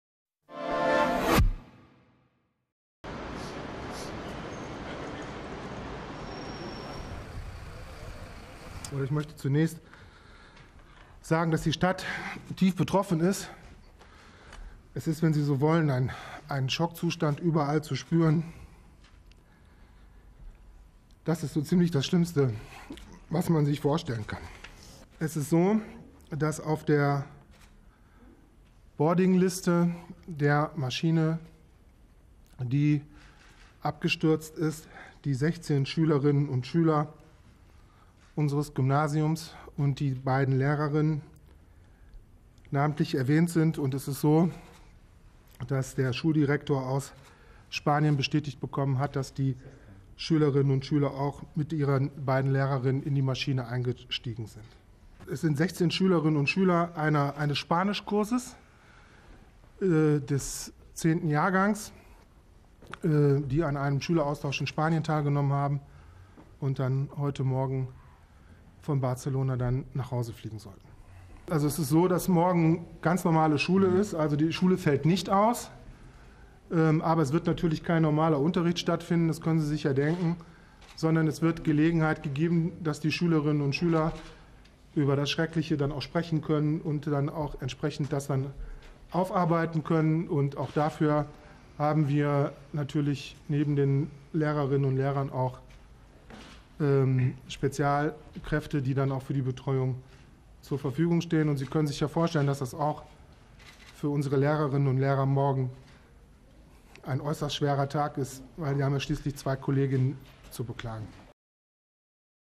Sixteen German pupils were on Airbus A320 flight
Mayor of Haltern in Germany confirms that 16 exchange students and two teachers boarded the Germanwings flight that crashed in the French Alps on Tuesday